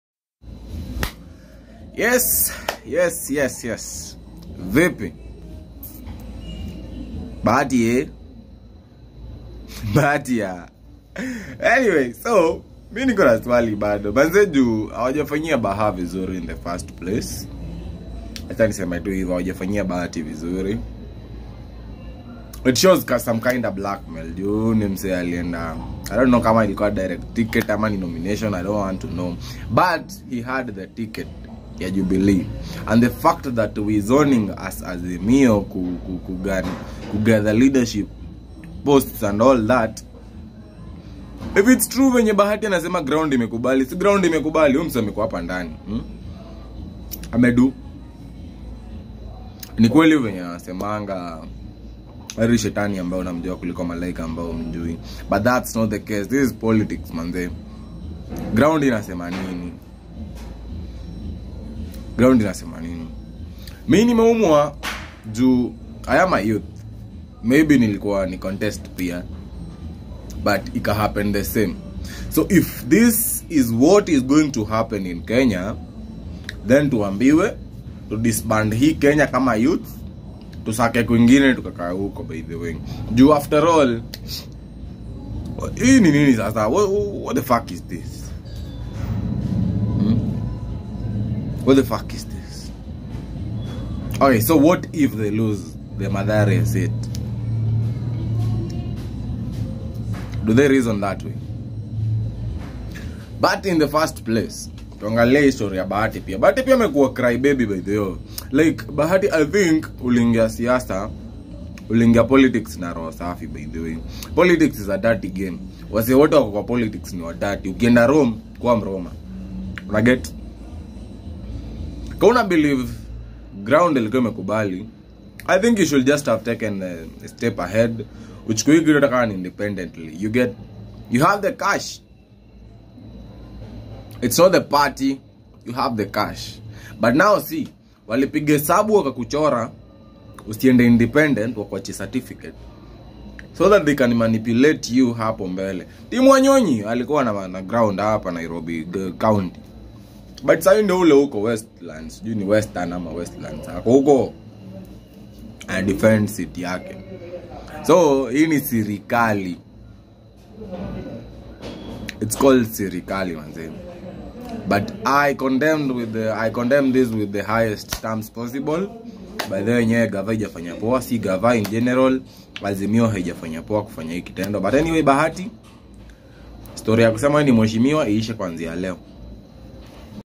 BAHATI mtoto wa mathare🥲🥲has been betrayed by his own Party.
Azimio has confirmed that Bahati will not be going on with his campaign for Mathare constituency..stepping down for His Odm counterpart.😥😥🥲